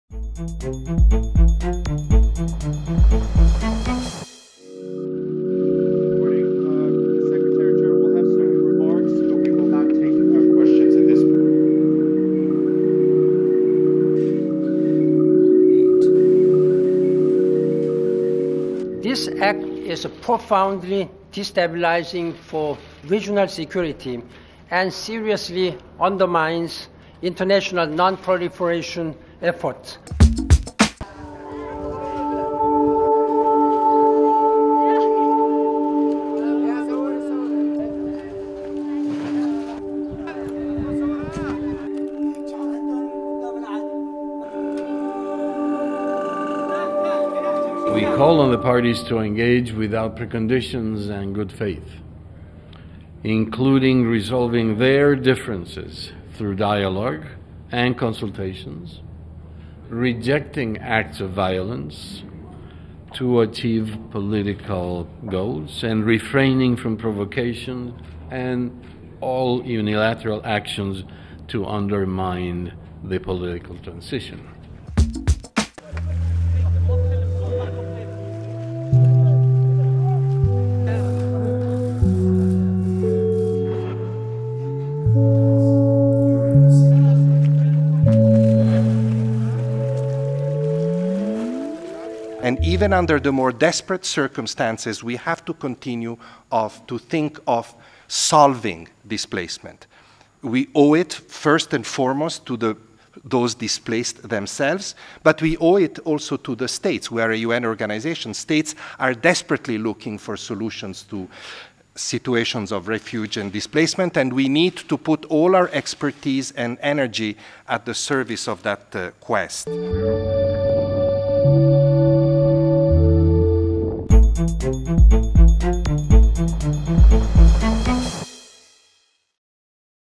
Resumo semanal da ONU em imagens #71
O secretário-geral da ONU, Ban Ki-moon, condenou o anúncio de testes nucleares pela Coreia do Norte; ONU pediu cessar-fogo no Iêmen; e o chefe da Agência da ONU para Refugiados pediu mais esforços diplomáticos para melhorar a assistência aos refugiados – estes são os destaques do resumo semanal da ONU em imagens.